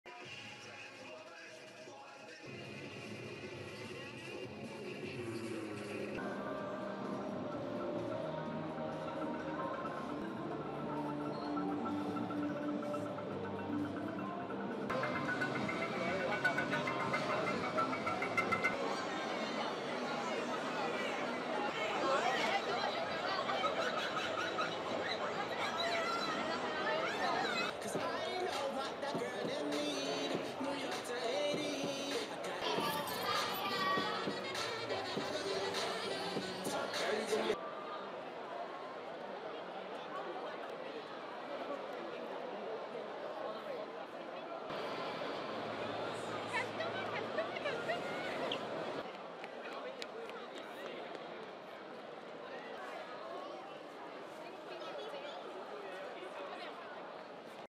Jeux vidéos et cosplay au salon Ani-Com de Hong Kong
Des passionnés de jeux vidéos et de cosplay se sont rassemblés au salon Ani-Com à Hong Kong. IMAGES ET SONORES.